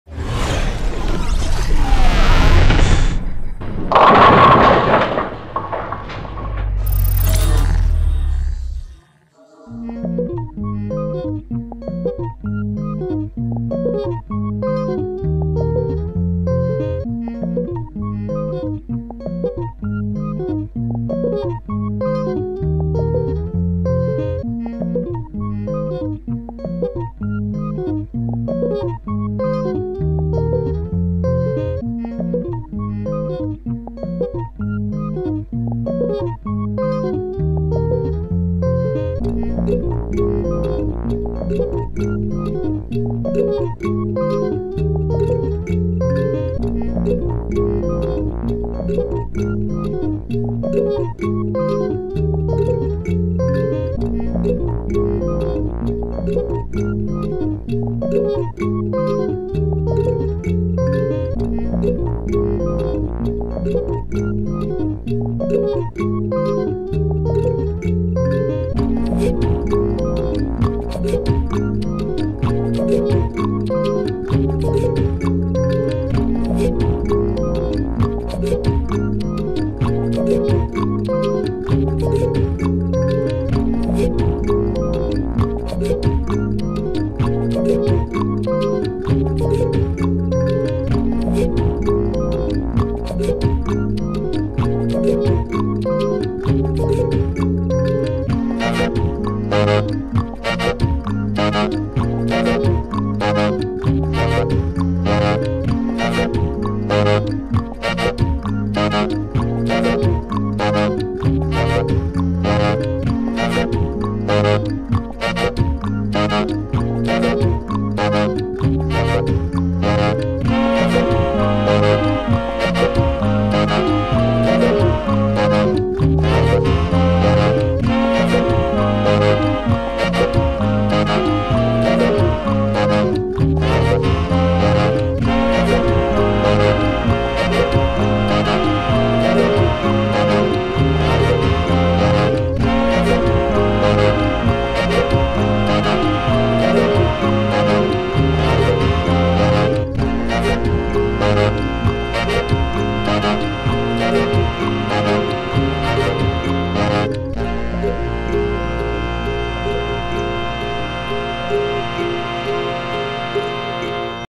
Making of - Squeezy Time Lapse
This is a "how is made" for a animation we did for Squeezy Sport Nutrition Romania . Have fun watching !